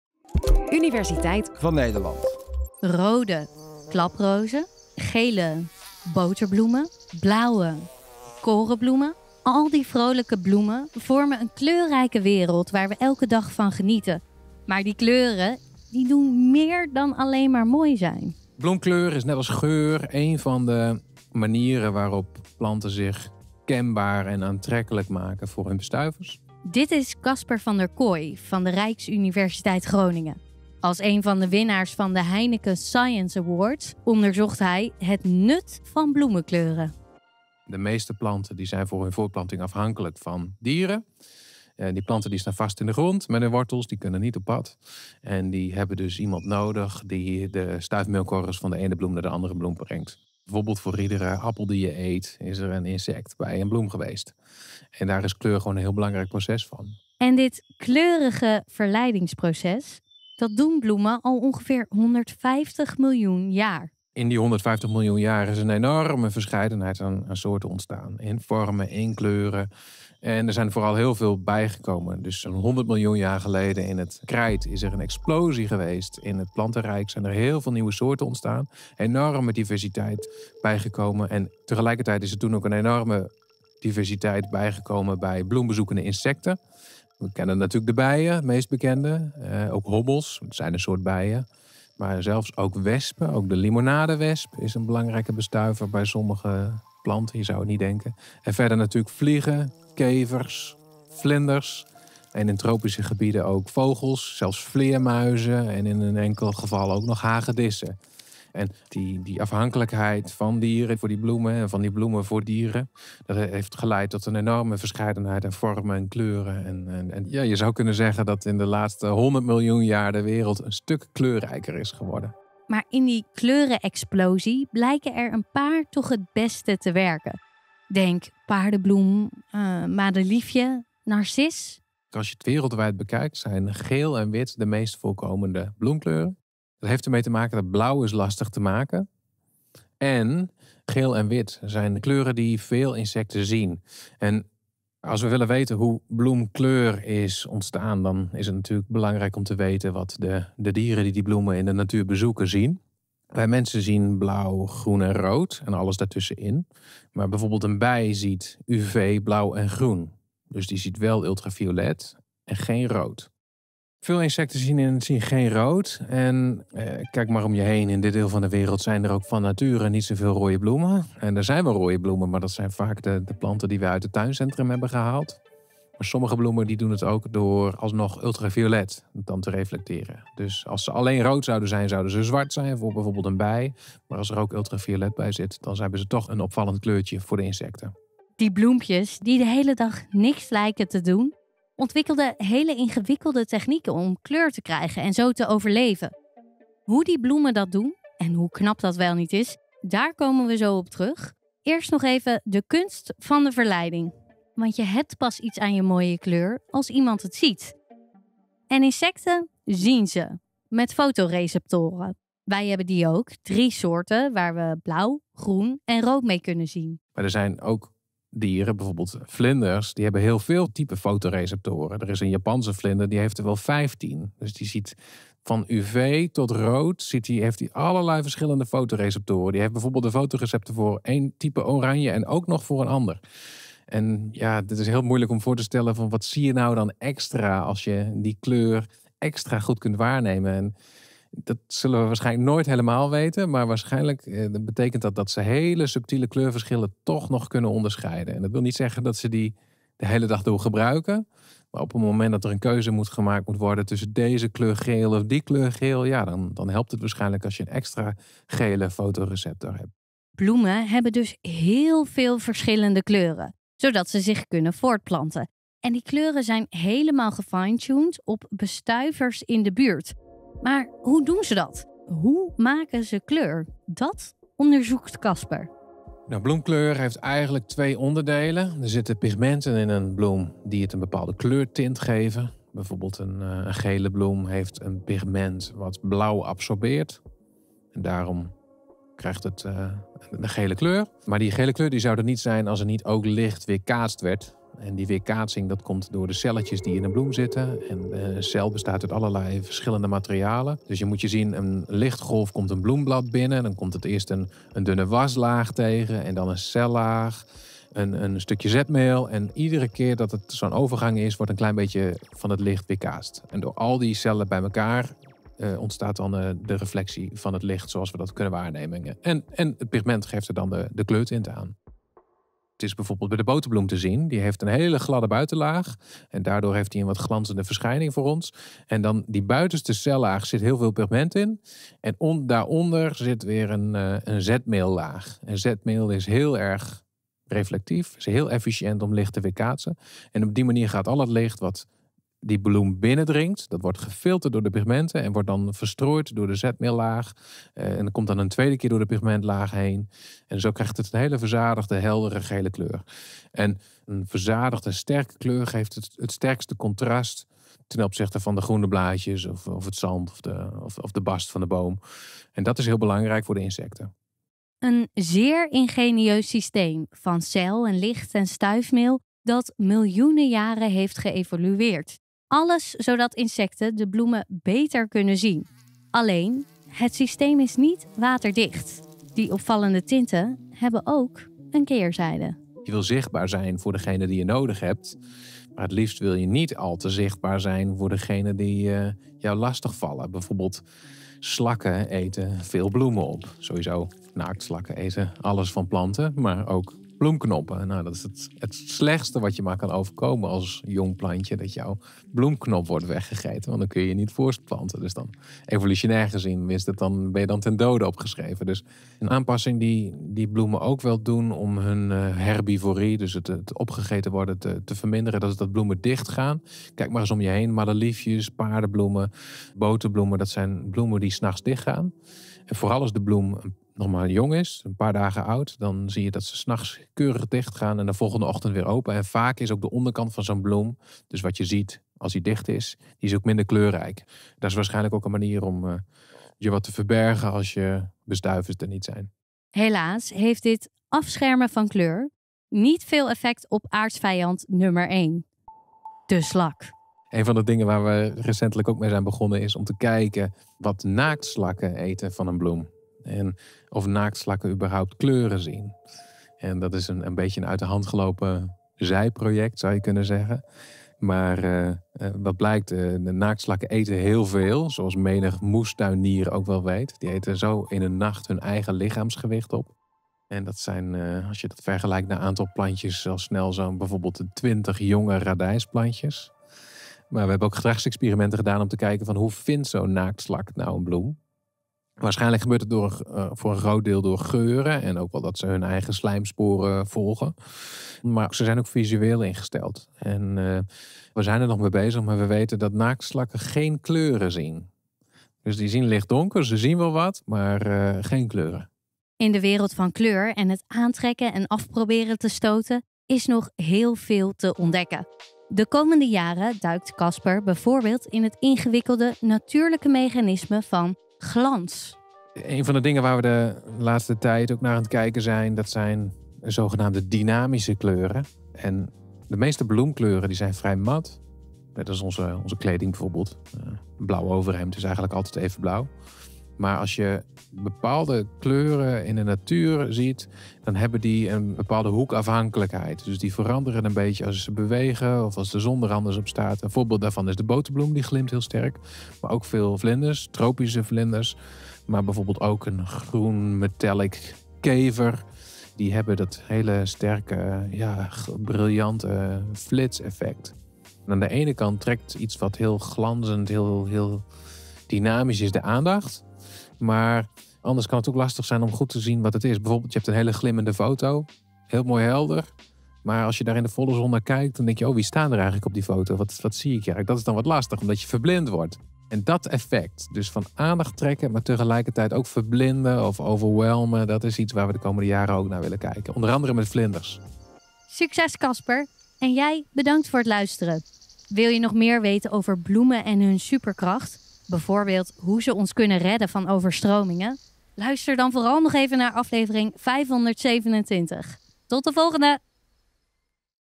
🎧 Waarom zijn bloemen bijna nooit blauw?
Casper van der Kooi (Rijksuniversiteit Groningen), winnaar van de Heineken Young Scientists Award, is gefascineerd door de kleurrijke wereld van bloemen. Elke bloem heeft weer een andere prachtige kleur. Casper onderzoekt hoe deze bloemen aan hun kleuren komen. En hoe ze daarmee de aandacht van bestuivers trekken. Welke kleuren hebben meer kans van slagen op het krijgen van nakomelingen?

Universiteit van Nederland komt naar Carré! Op 28 oktober vieren we onze 10 verjaardag met een speciale voorstelling: Wat als de tijd stopt? Tickets